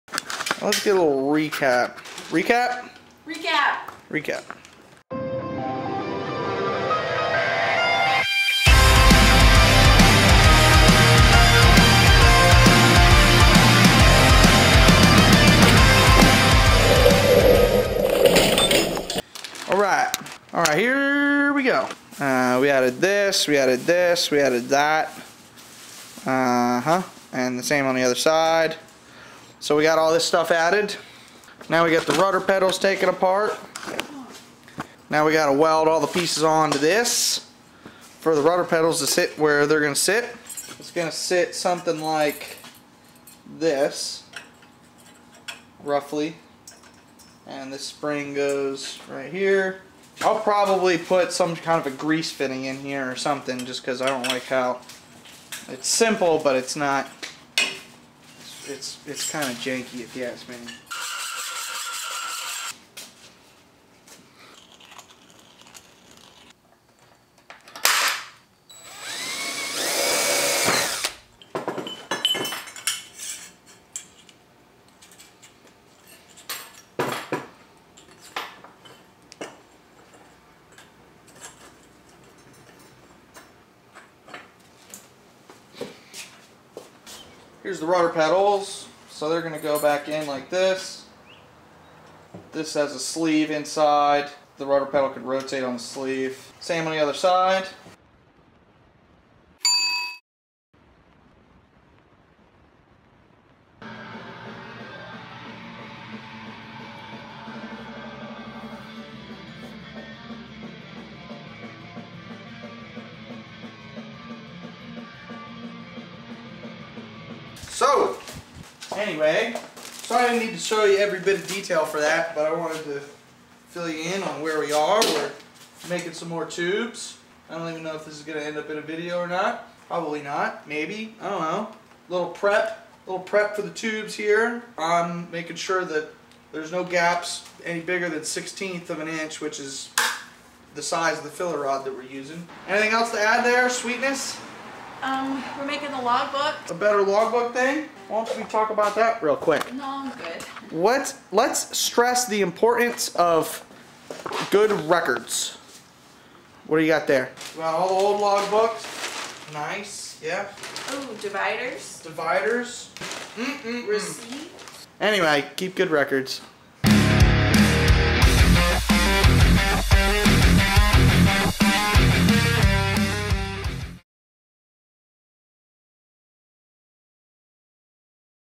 Skybolt Episode 8 Rudder Pedals
DG Fabrication Series "The Raven" by Austin Daniel. This Steen Skybolt aerobatic biplane is getting a landing gear upgrade. The series will highlight many of the key parts of the repair and modification process to adapt the Skybolt to the Grove aluminum spring landing gear.